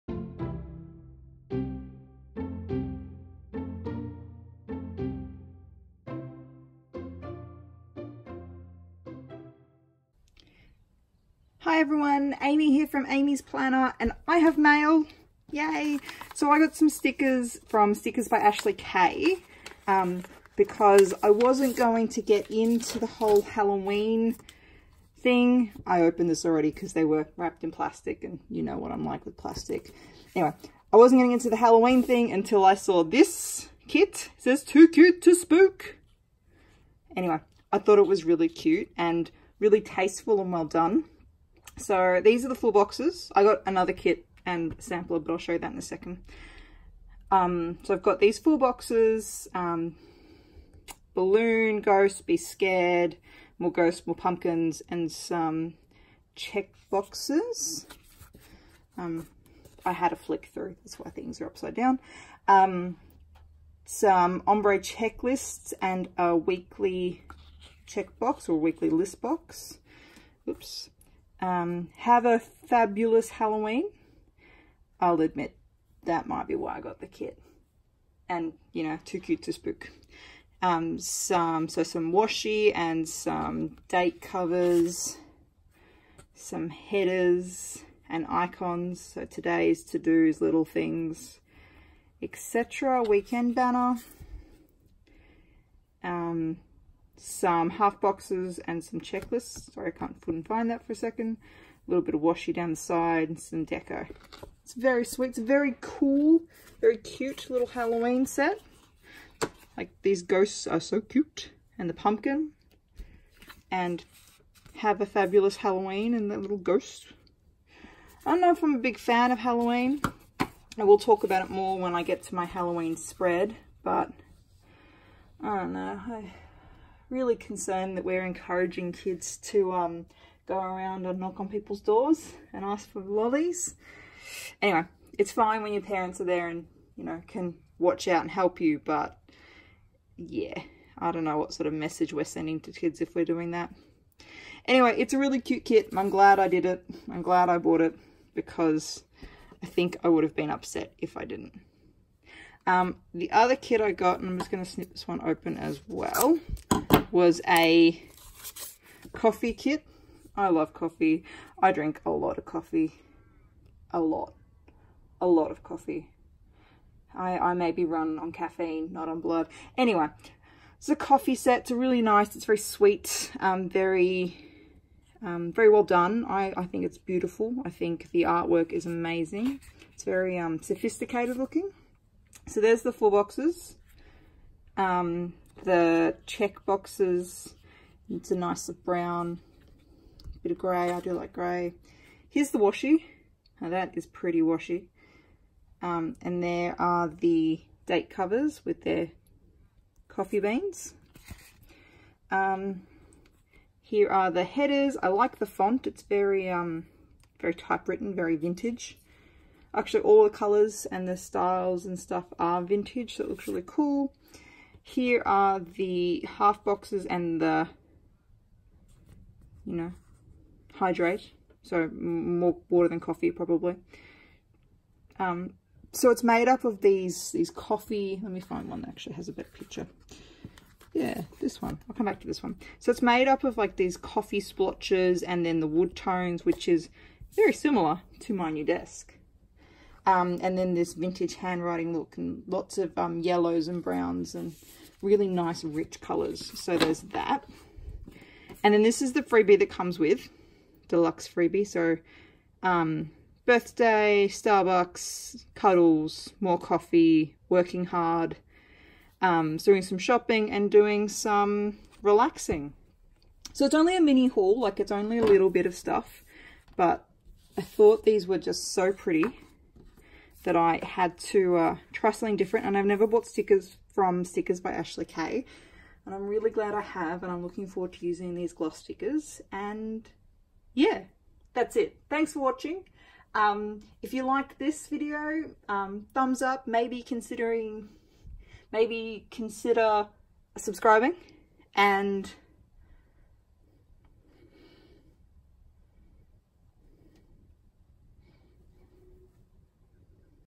Stickers by Ashley K Haul
Hi everyone,

I found these amazing kits at Stickers by Ashley K and they are wonderful. Have a look!

You can find me over on Instagram at @amysplanner